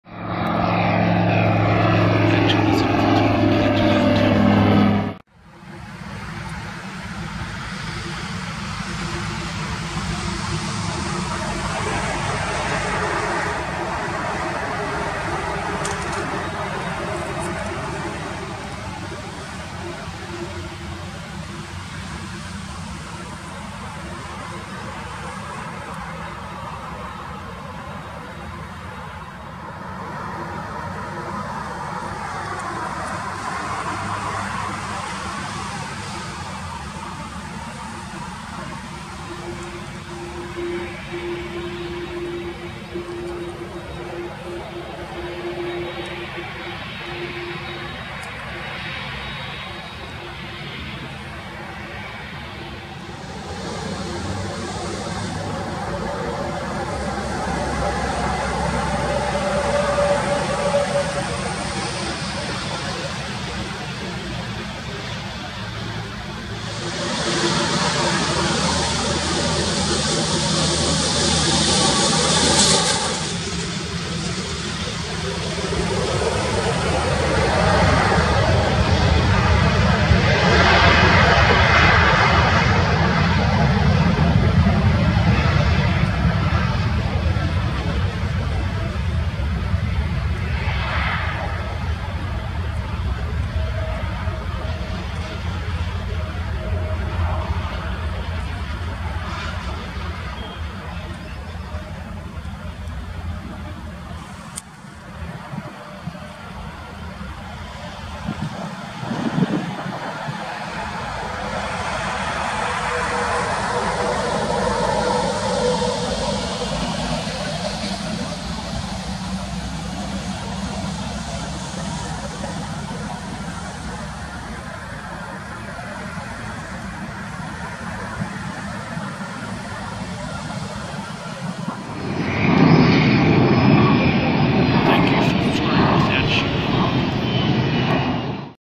Cathay Pacific Cargo Boeing 747-867F Depature from Frankfurt Airport [Close-up]
Flight:
- Flight Number: CX64
- Depature Airport: Frankfurt International Airport, Germany (EDDF/FRA)
- Arrival Airport: Amsterdam Schipol International Airport, Netherlands (EHAM/AMS)

Aircraft:
- Manufacturer: Boeing Commercial Airplanes
- Type: 747-867F
- Built: 2013
- Serial Number: 43536
- Registration: B-LJL
- Operator: Cathay Pacific Airways Limited

Camera: Nikon Coolpix B700
Edited with: MAGIX Video Pro X